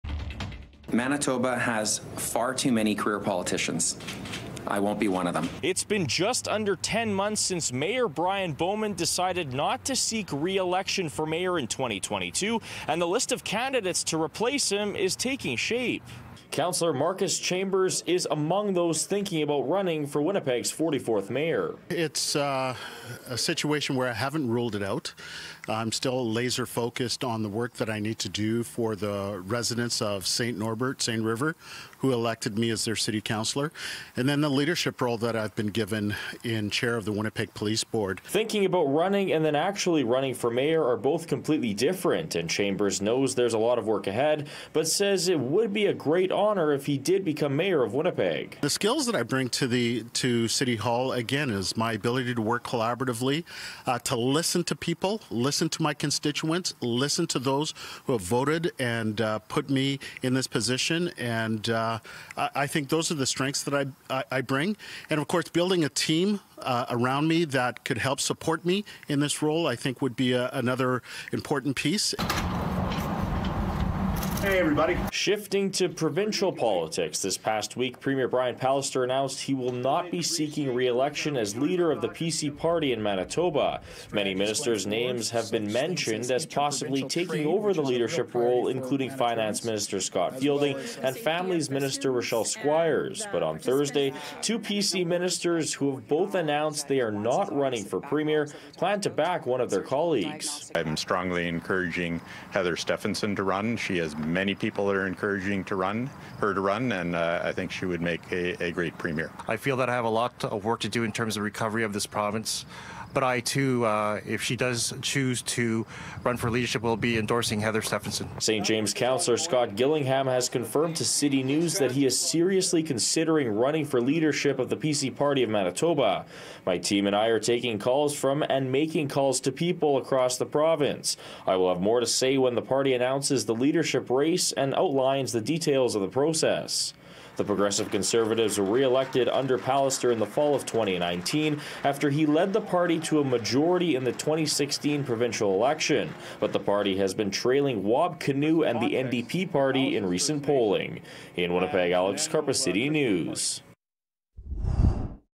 Candidates for Winnipeg’s Mayor and Manitoba’s Premier emerge
Both the Mayor of Winnipeg and the Premier of Manitoba have announced they won’t be seeking re-election and the list of candidates are already starting to appear. Alex Karpa reports.